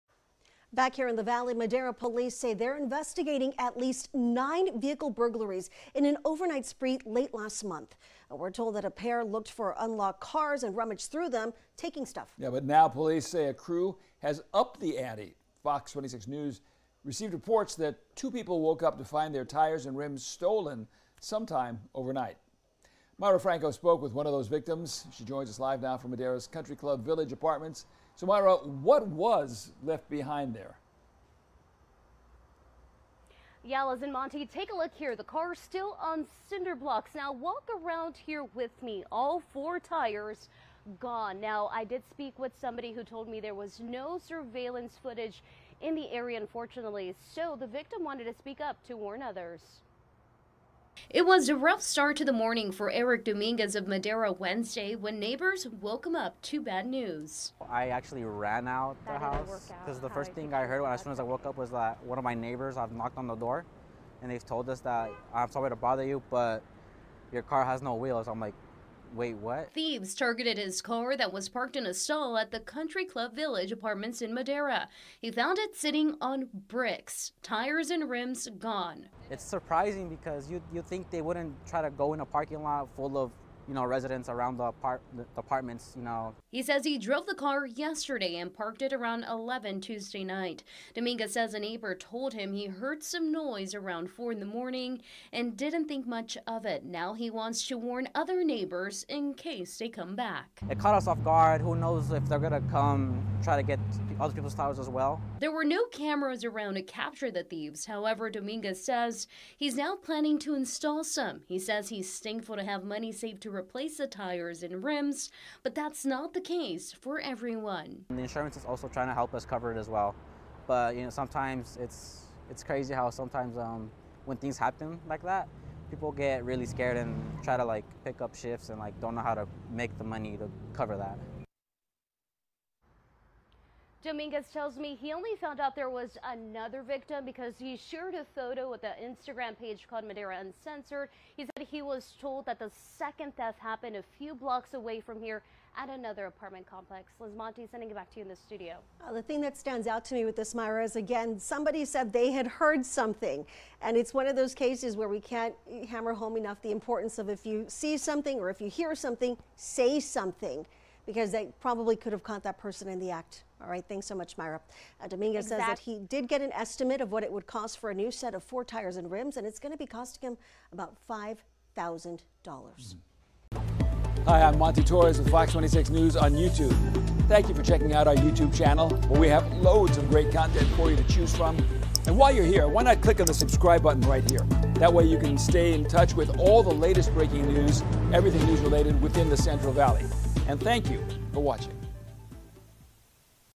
Tires stolen from parked car at Madera apartment complex
A man in Madera, California says he was woken up by neighbors knocking at his door alerting him that his car’s tires had been stolen.